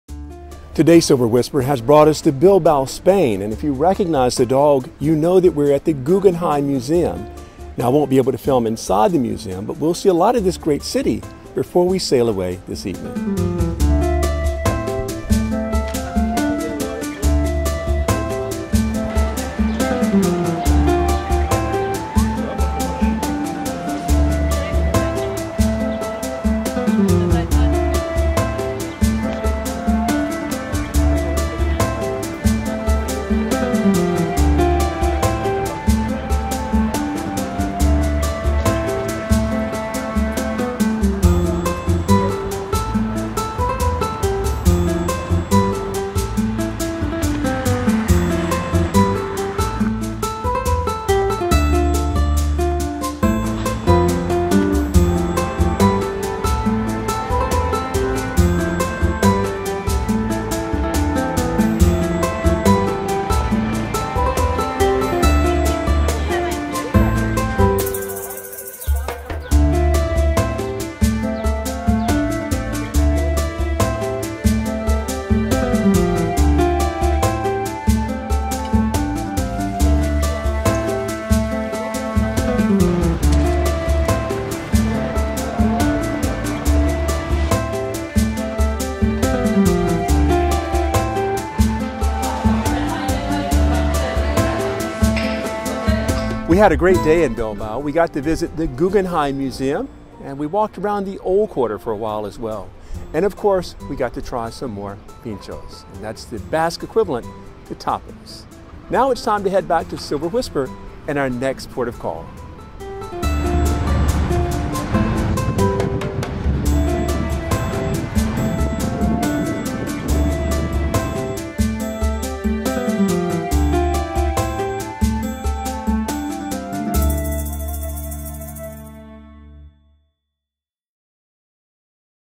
On Silver Whisper: A Special Shore Excursion To Bilbao & The Guggenheim
This morning, Silver Whisper docked at the Port of Bilbao, about 20 minutes from the city center of Bilbao and its famed Guggenheim Museum. Here are the highlights from our day.